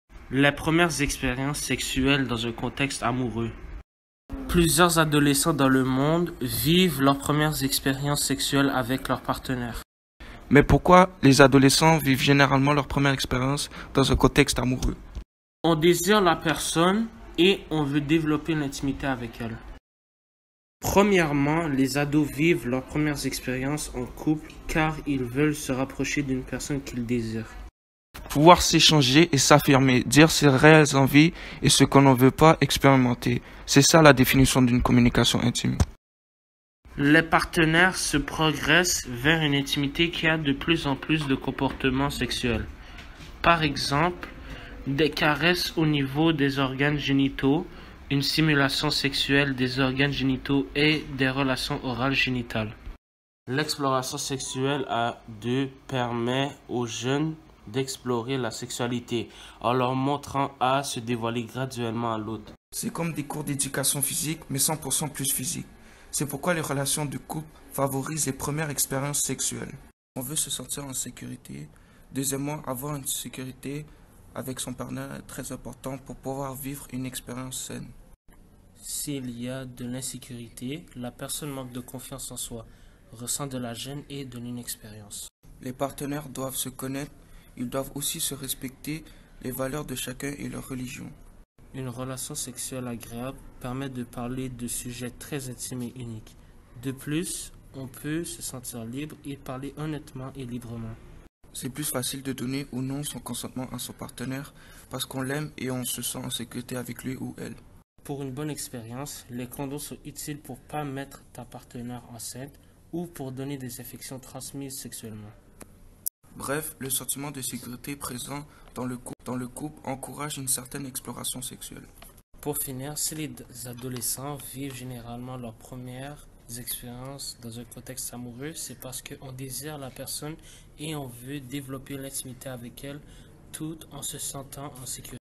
Les premières expériences sexuelles dans un contexte amoureux
Cette capsule explicative a été réalisée par des élèves en classe de français de 3e secondaire de l'école Henri-Bourassa. Ils ont relevé le défi d'expliquer pourquoi les adolescents vivent généralement leur première expérience sexuelle dans un contexte amoureux sur un ton humoristique.